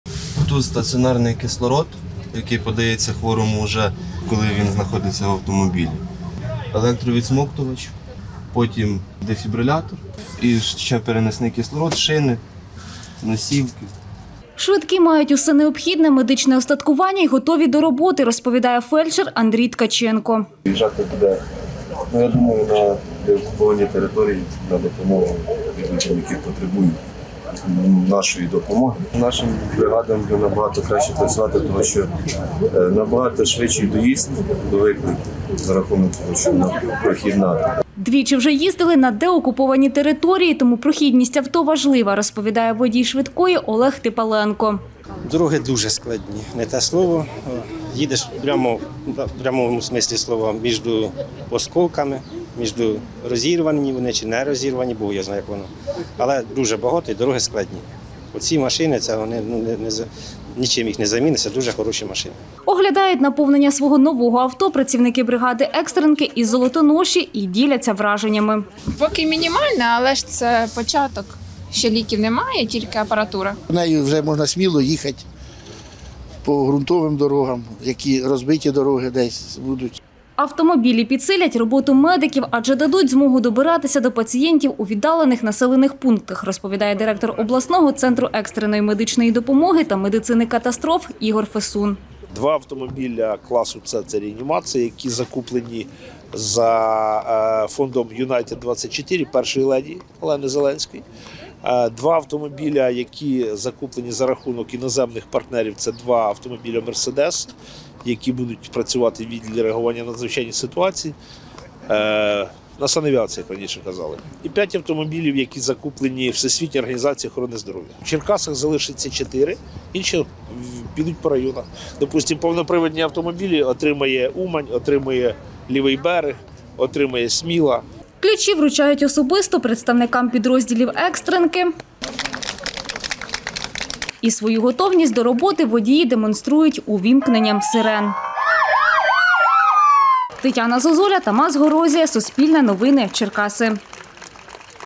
Черкаська екстренка отримала дев’ять нових автомобілів
Дев’ять нових автомобілів отримав Черкаський обласний центр екстреної меддопомоги. Таке підсилення цьогоріч отримують вперше. Їхня вартість складає близько 20 мільйонів гривень. Це автомобілі класу «С» та повнопривідні позашляховики, які в подальшому будуть виїжджати і на деокуповані території України.
Авторка - Тетяна Зозуля,
Оператор - Тамаз Горозія